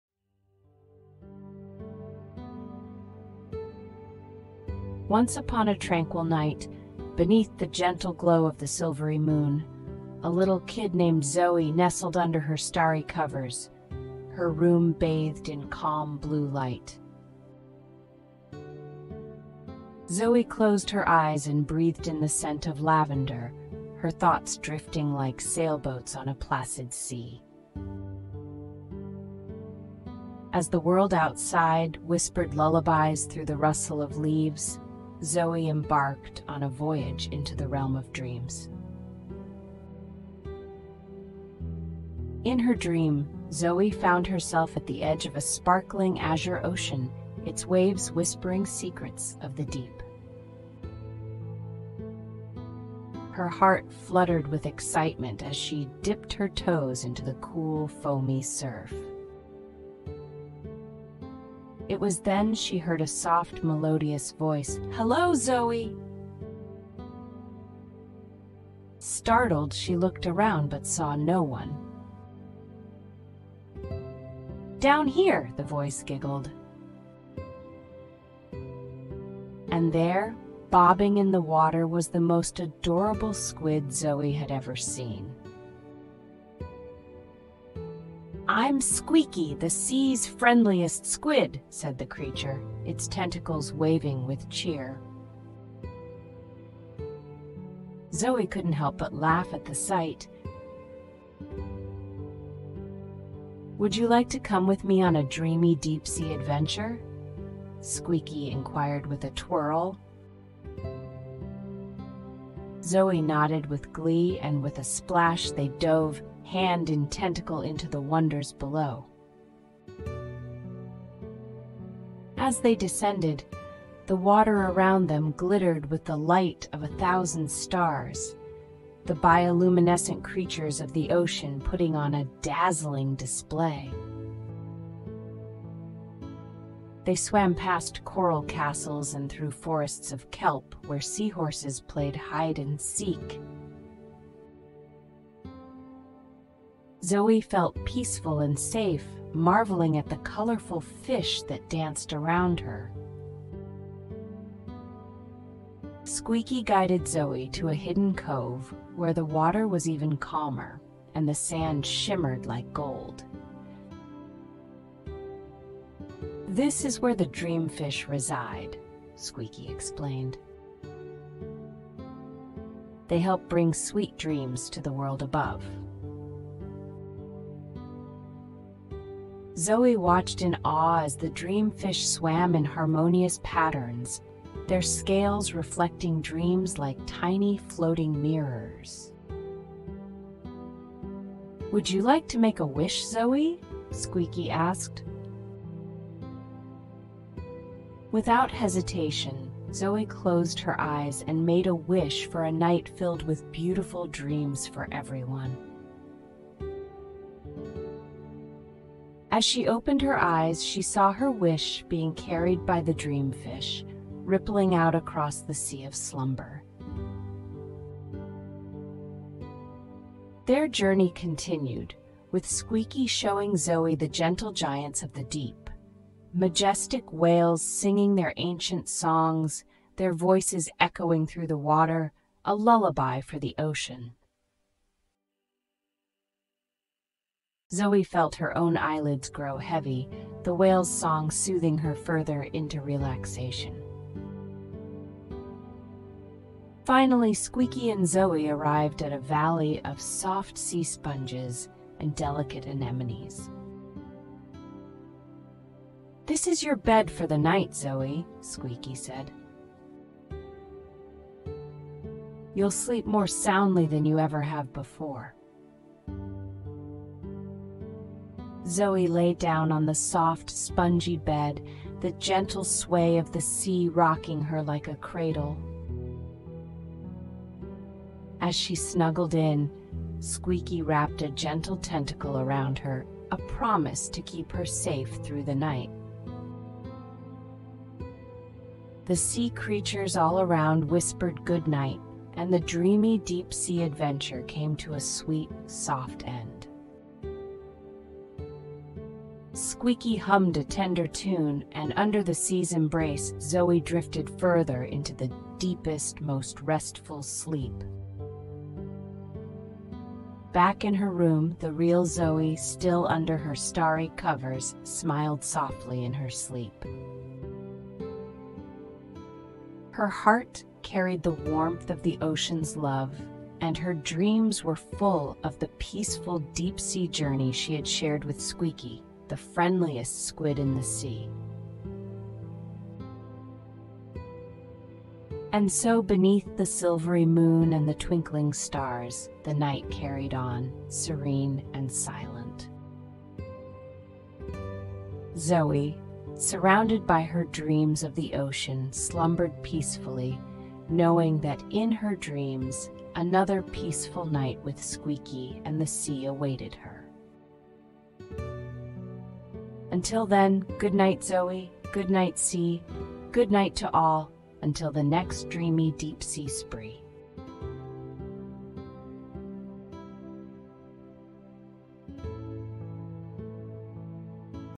Zoey's Dreamy Deep-Sea Adventure | Bedtime Story For Kids in English
Welcome to the bedtime story tonight! Tonight, we are going to tell a story about Zoey experiences a serene underwater adventure in her dreams, where she meets Squeaky, the friendliest squid, and explores the wonders of the ocean. Her dream is filled with magical creatures and ends with Zoey making a wish with dreamfish, culminating in the most restful sleep both in her dream and in reality.